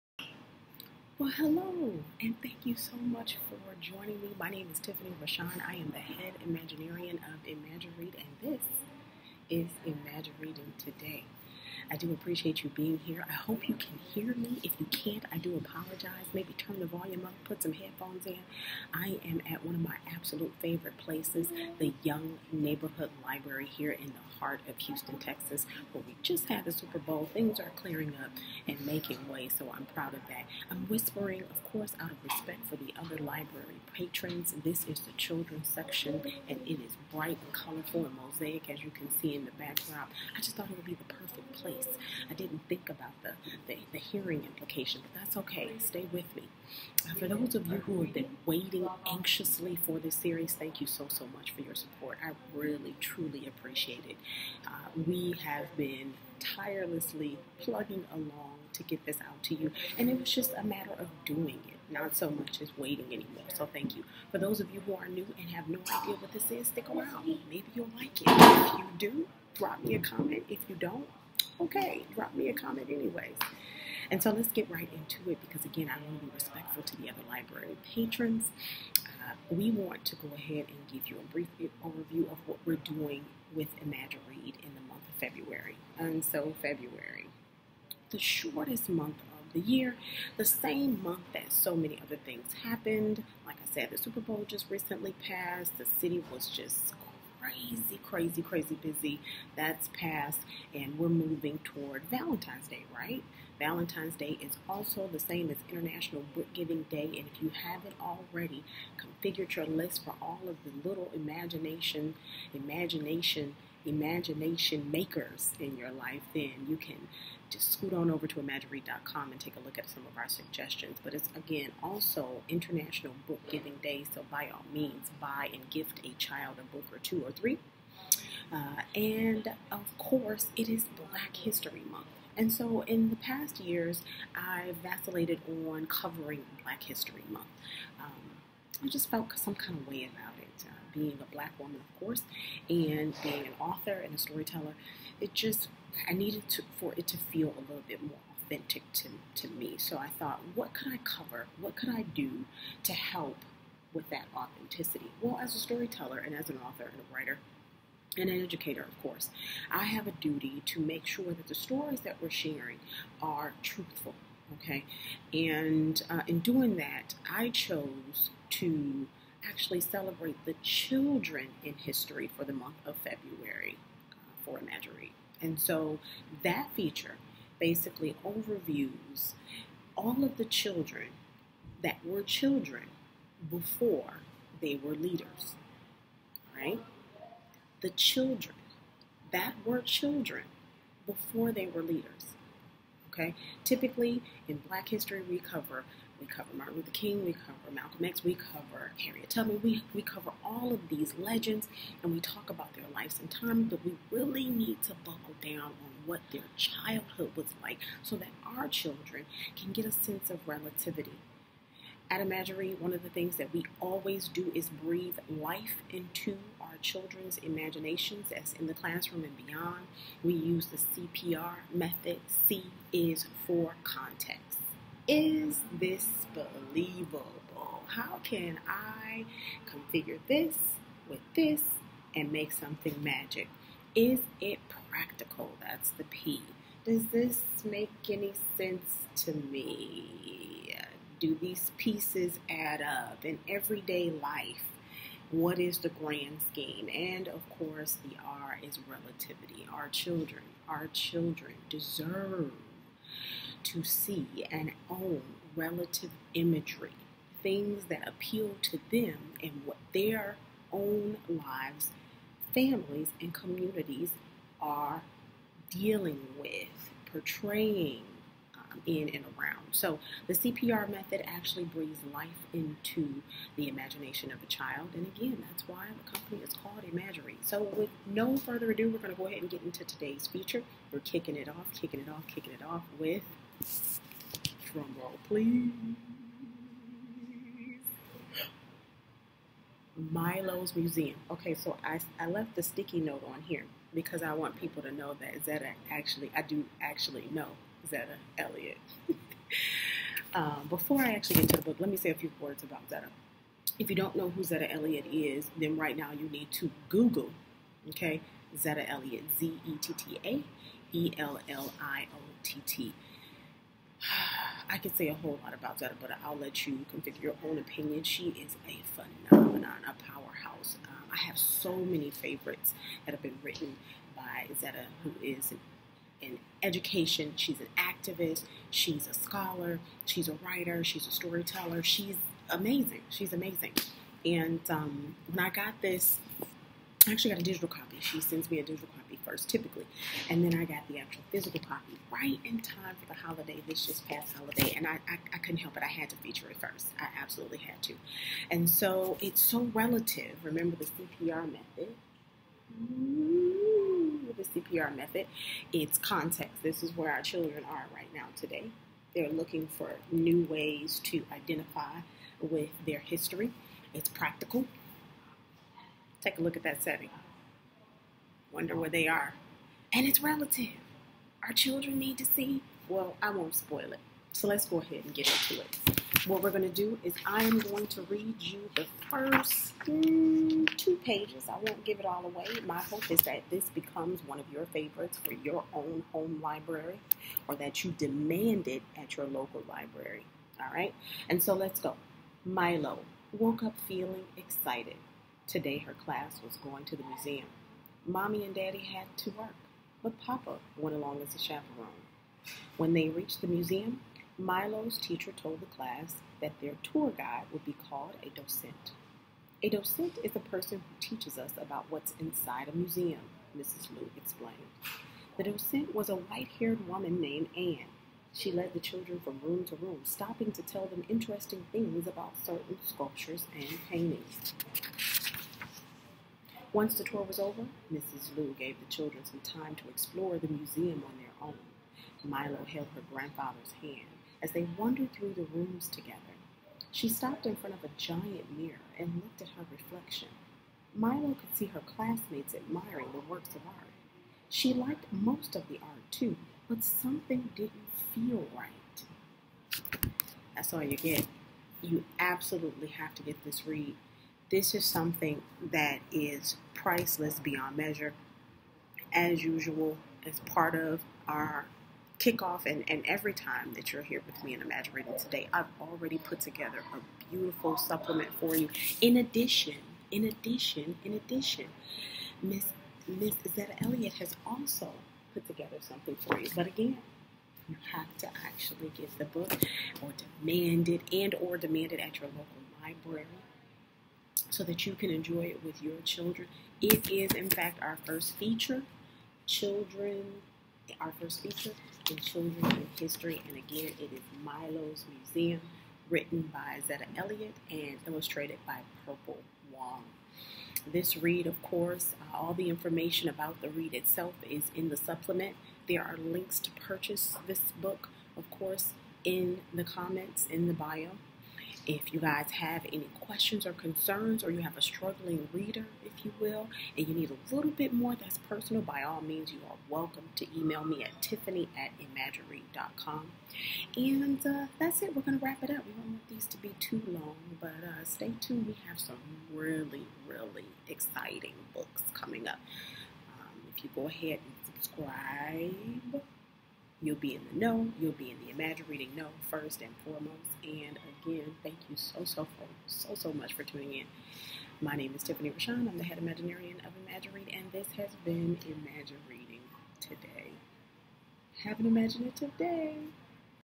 Imagireading Today! Premier featuring Milo's Museum
Join Tiffany Rachann as she premieres the series with the perfect read for Imagiread's Children in History feature, Milo's Museum, at the Young Neighborhood Library. SEE LINKS